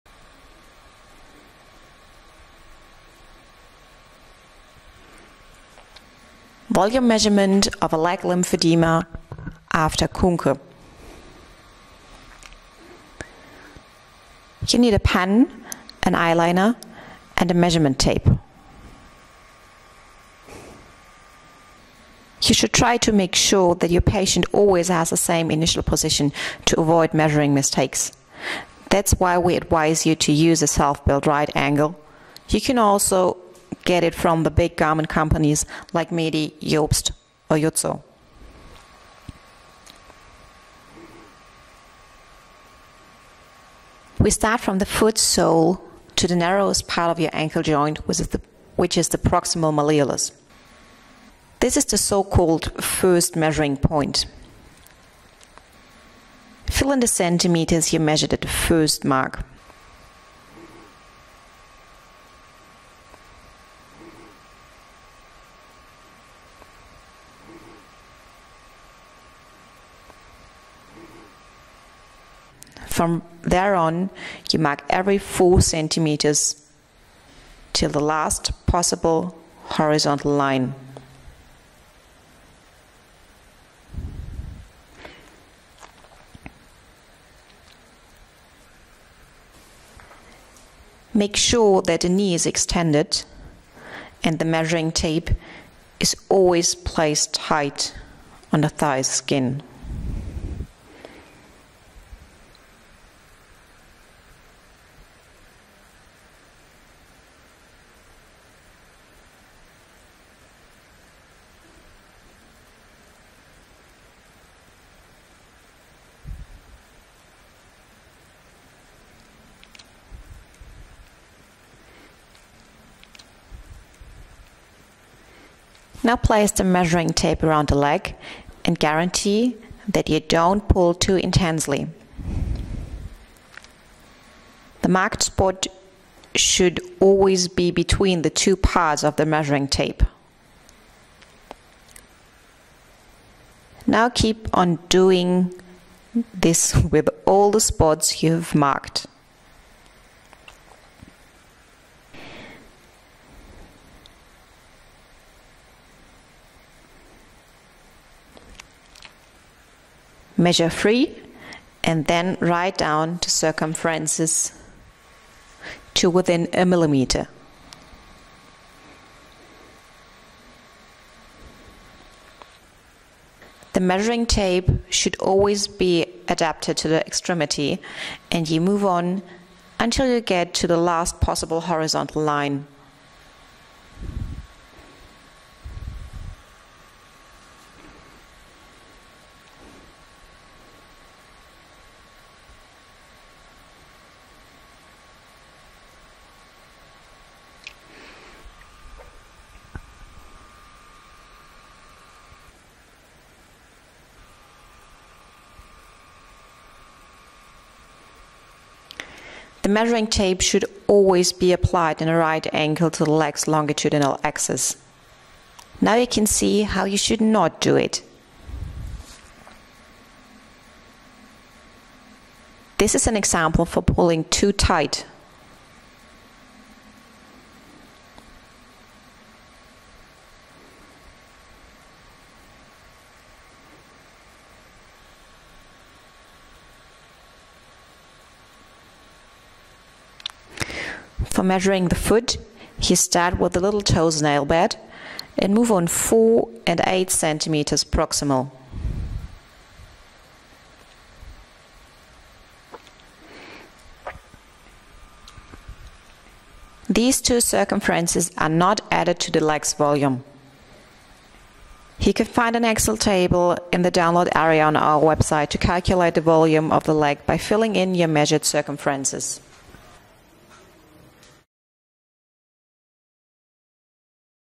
measurement of a leg lymphedema patient
This movie is showing how to measure a leg lymphedema patient with the Kuhnke method in the lymphedema clinic Wittlinger. It allows you to calculate the volume exactly to ml.